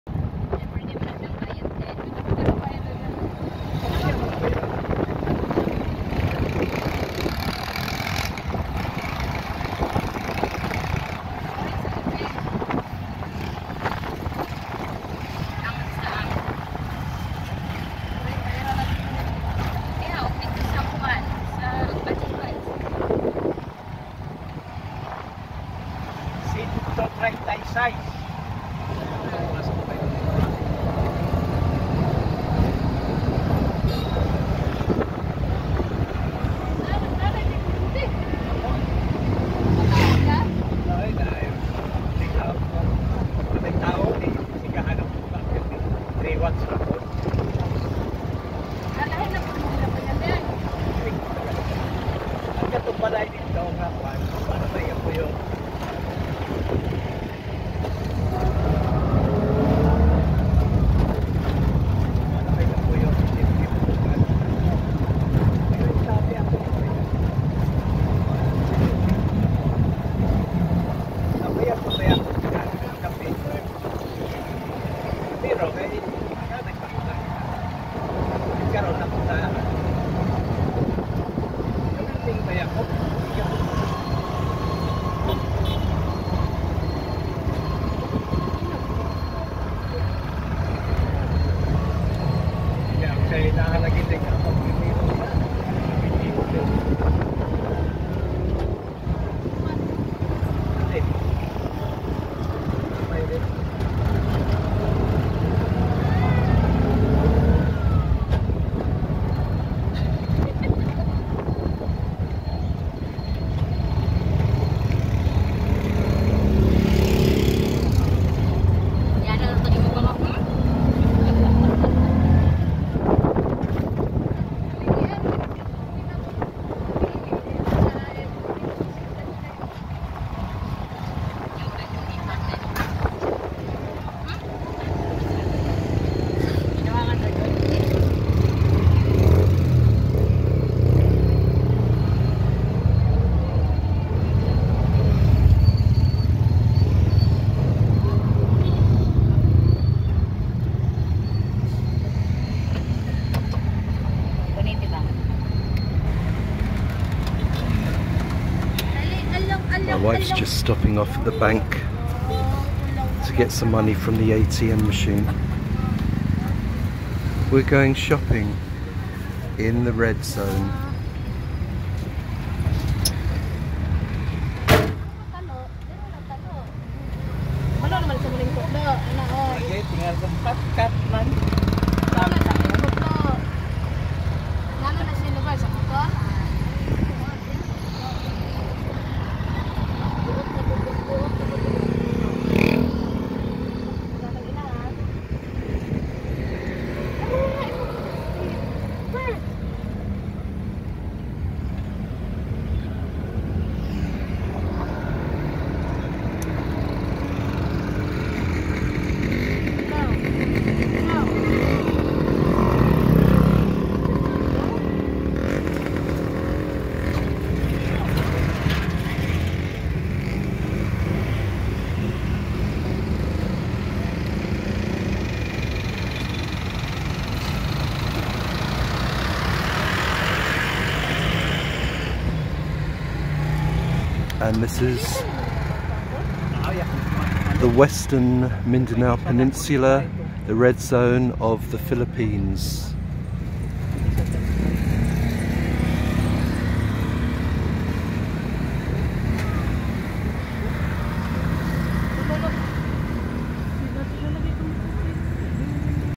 PHILIPPINES EXPAT IN THE RED ZONE OF MINDANAO PHILIPPINES
Expat living in the Philippines going to town in the red zone of Western Mindanao